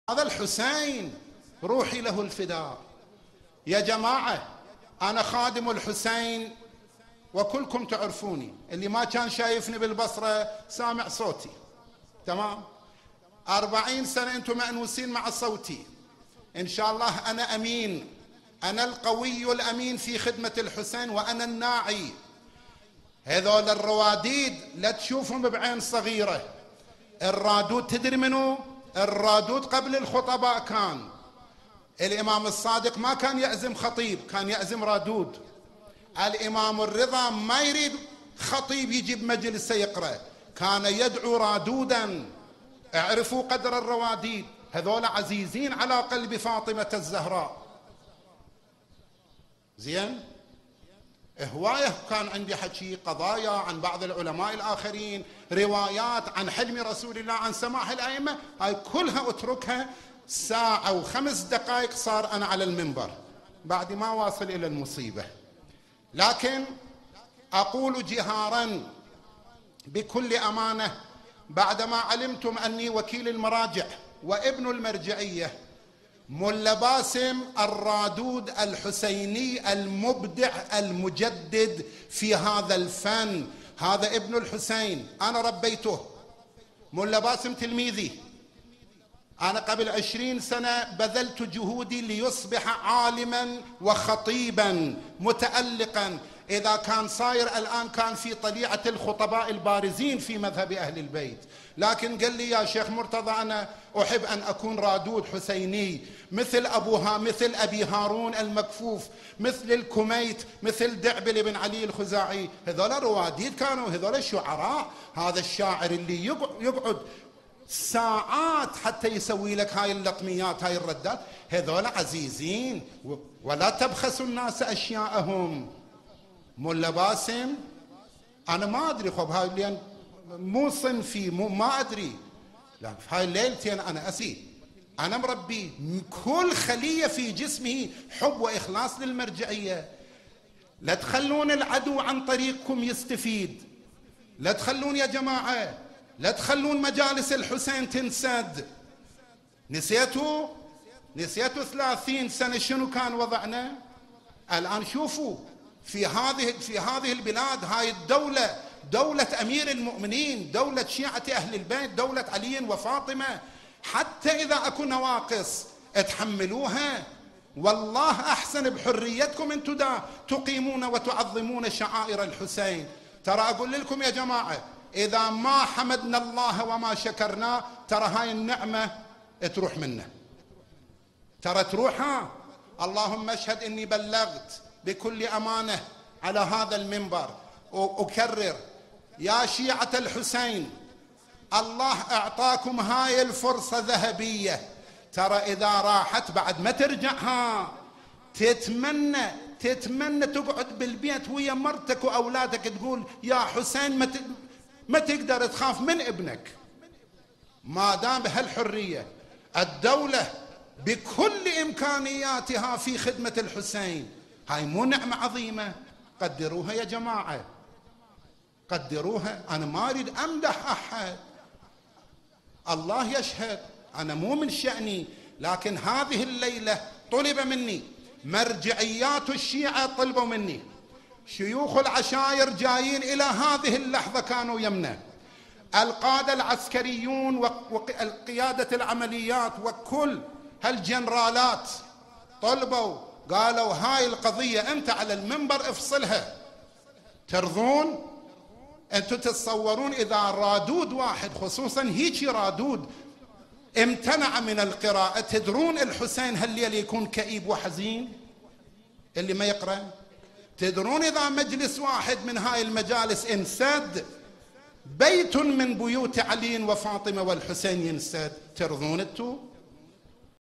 المرجعية مع باسم الكربلائي
موضوع  الحاج باسم الكربلائي الحسيني الزهرائي
 كشف هوية الكثير من البيجات السنية الداخلة بأسم التشييع وتتحكم بالرأي العام الشيعي
وكشف المنافىقين بين الشيعة ممن في قلوبهم حب العىجل والطاغىوت